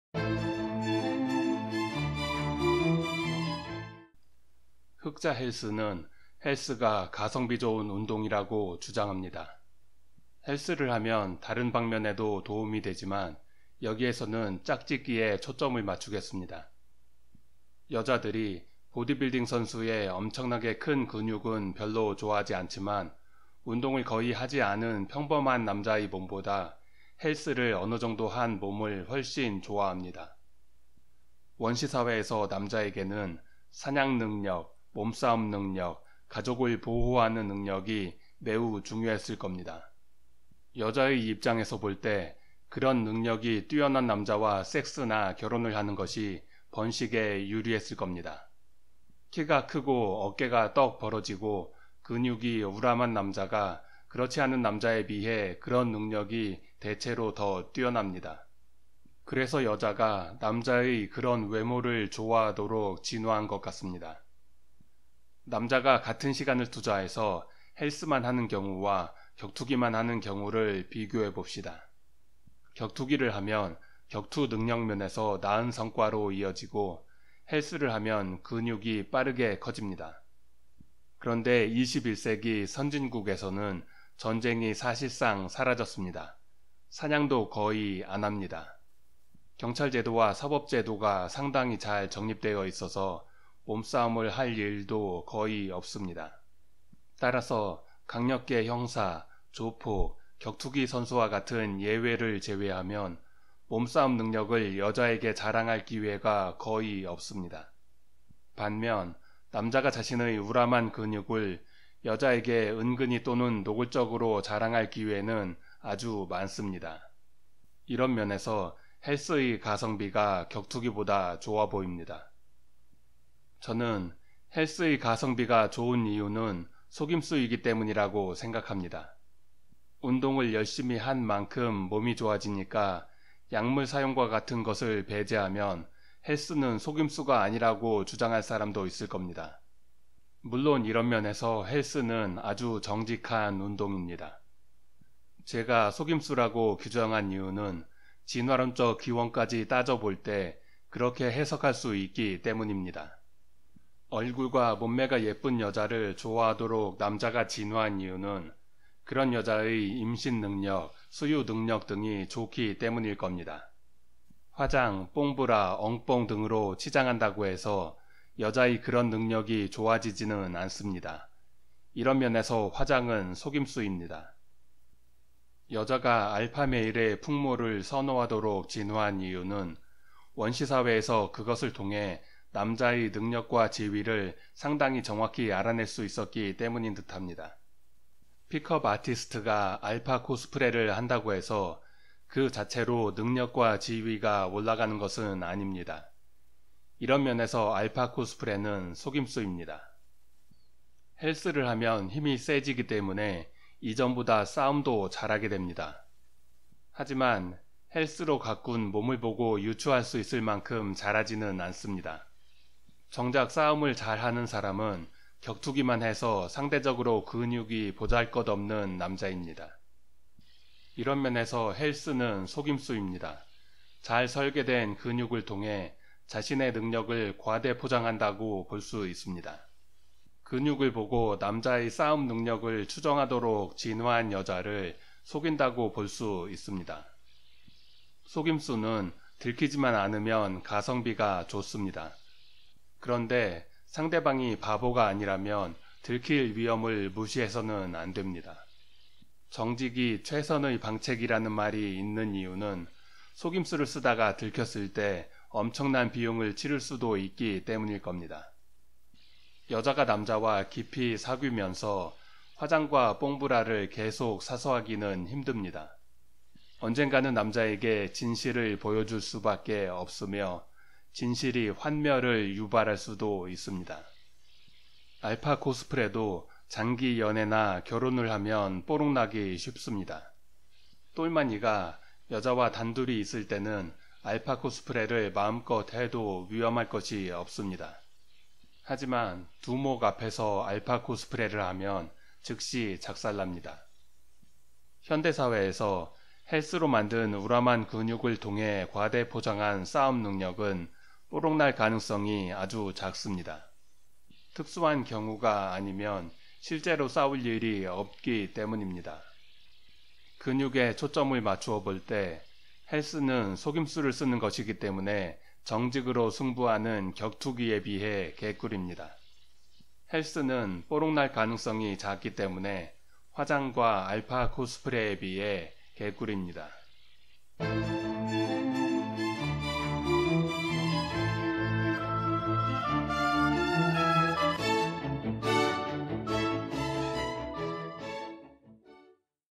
연애심리 진화심리 56. 헬스가 격투기보다 남자에게 개꿀인 진화심리학적 이유 (흑자헬스는 기분 나쁘겠지만 흑자헬스가 옳다)
흑자헬스는 격투기에 비해 헬스의 가성비가 더 좋다고 주장합니다. 저도 어떤 면에서는 그렇다고 생각합니다. 이 영상에서는 짝짓기에 초점을 맞추어서 헬스가 왜 남자에게 개꿀인지 진화심리학 가설을 소개하겠습니다.

여자는 키, 어깨, 근육 등을 보고 남자의 사냥 능력, 몸싸움 능력, 가족 보호 능력을 추정하도록 진화한 듯합니다. 헬스를 통해 만들어낸 어느 정도 커다란 근육을 보고 여자들이 좋아하는 이유는 이 때문인 듯합니다.

그런데 헬스는 실제 능력보다 과장해서 근육을 보여줍니다. 이런 면에서 속임수라고 볼 수 있습니다. 그리고 현대 사회에서는 실제로 싸울 일이 거의 없기 때문에 이 속임수가 뽀록날 일도 거의 없습니다.